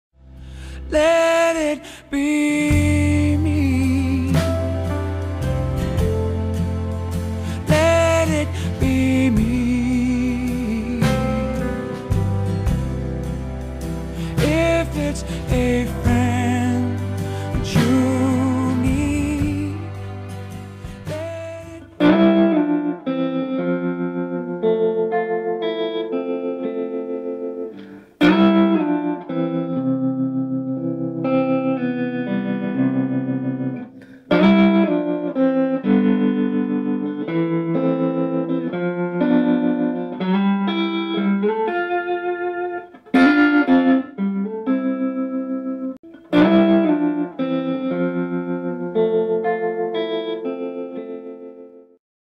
Vox AC15C1 tremolo w/ Telecaster
Little video with a Fender MIM Classic 50's Telecaster, TC Electronic Nova System with a light reverb and Vox AC15C1 tremolo on the Top Boost channel.

Sorry for the poor video and audio quality but I used my old Nokia N95 phone camera. The image stops sometimes but not the audio!